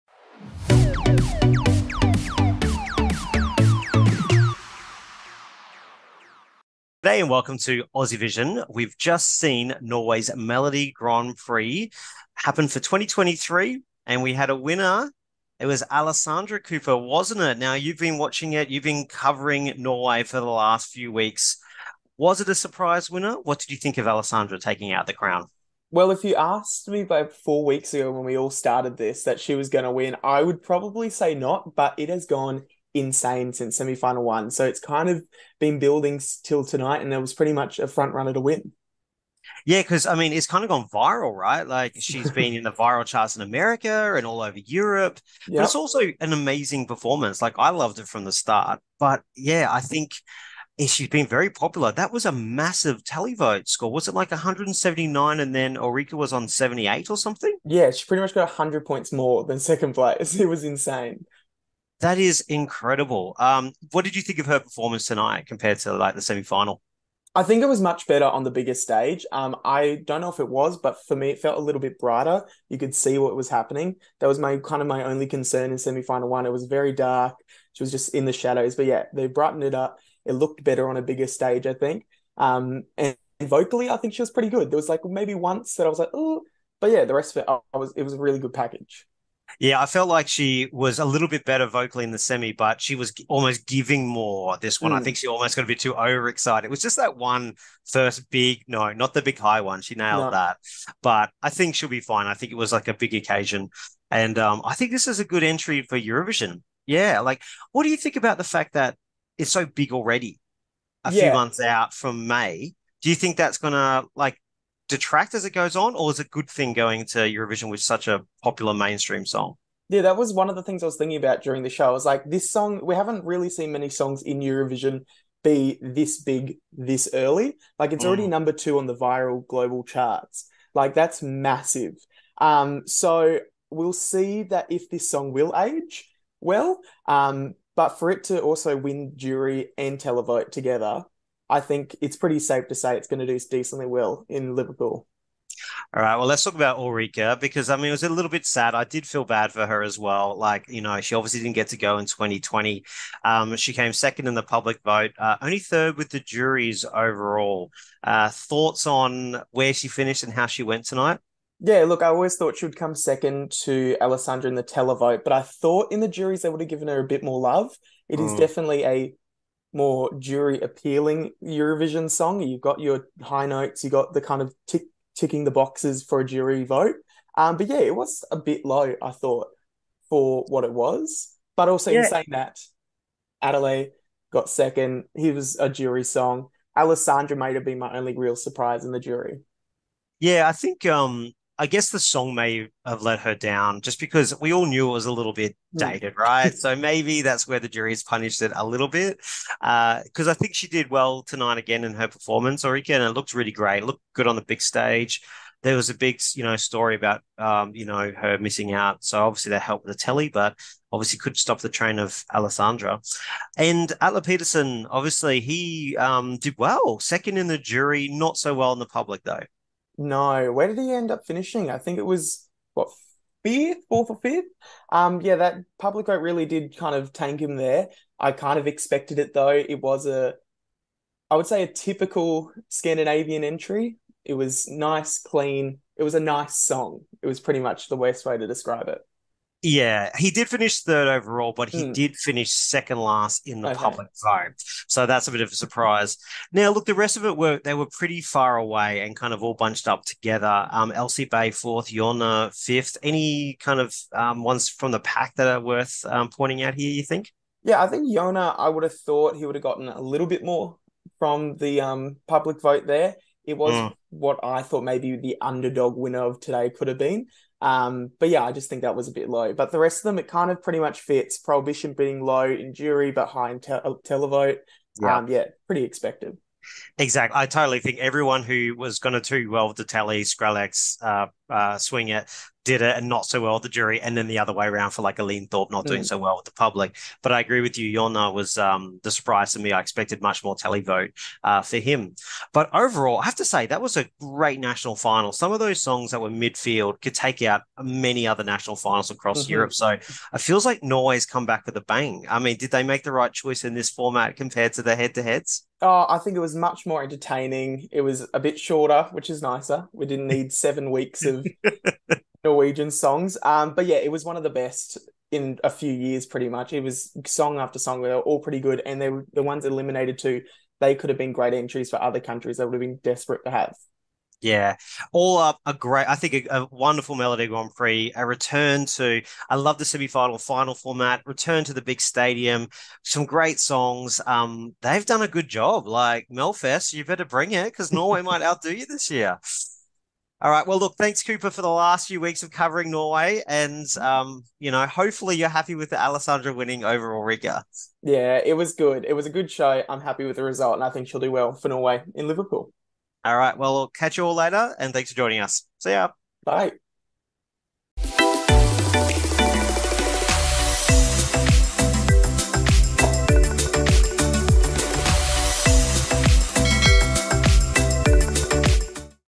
Eurovision: Norway MGP 2023 Wrap-up and Review
Cooper and Dale from the Aussievision team wrap-up the final of Norway's Melodi Grand Prix 2023.